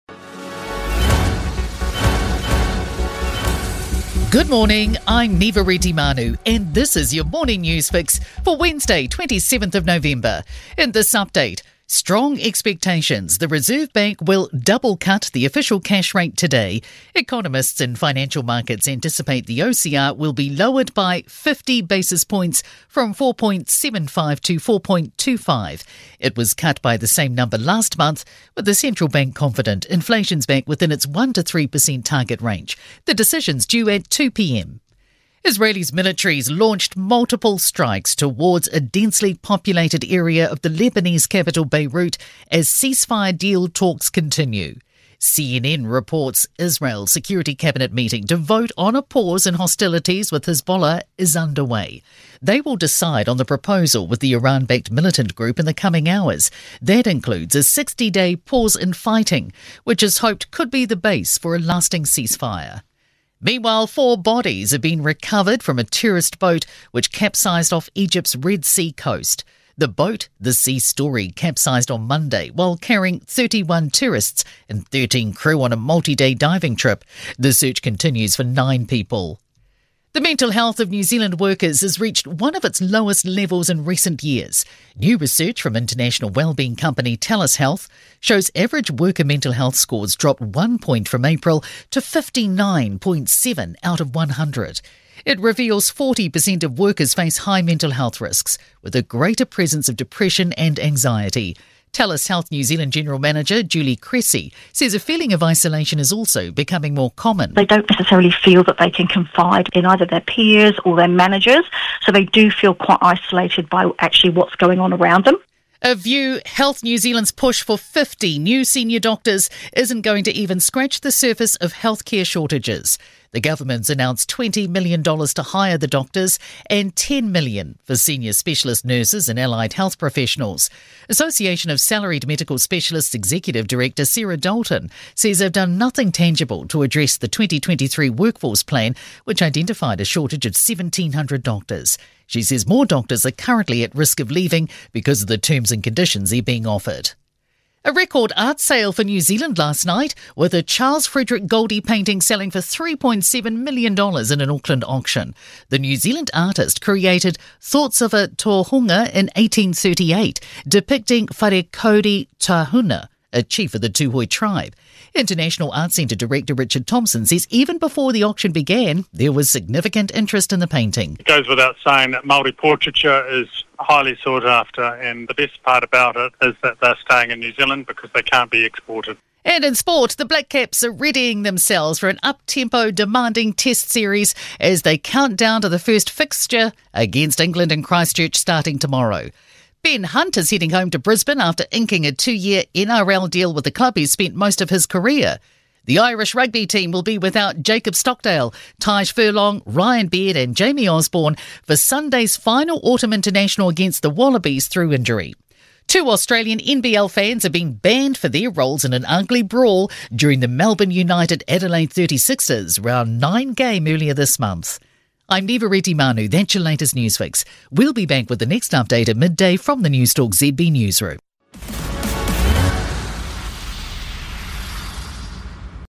Morning Edition: 27 November 2024
The Morning Edition of News Fix for 27 November 2024, straight from the Newstalk ZB newsroom - bringing you everything you need to know in news, politics, business, entertainment and sport.